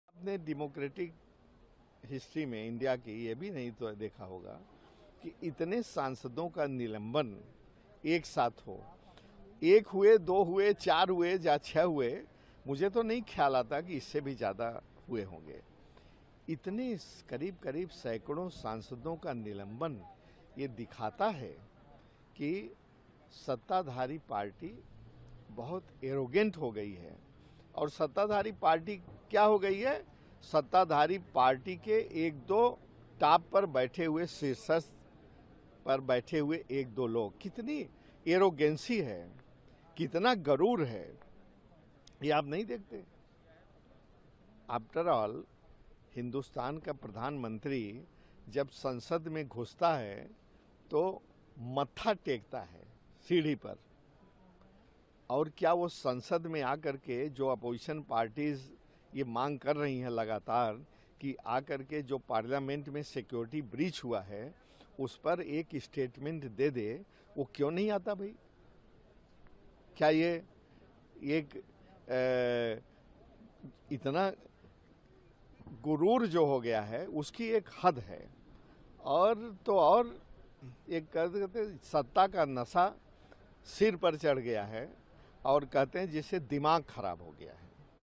141 सांसदों के निलंबन पर बसपा सांसद Shyam Singh Yadav का बड़ा बयान | Mayawati | BSP
141 सांसदों के निलंबन पर बसपा सांसद Shyam Singh Yadav का बड़ा बयान | Mayawati | BSP

हमसे जुड़े -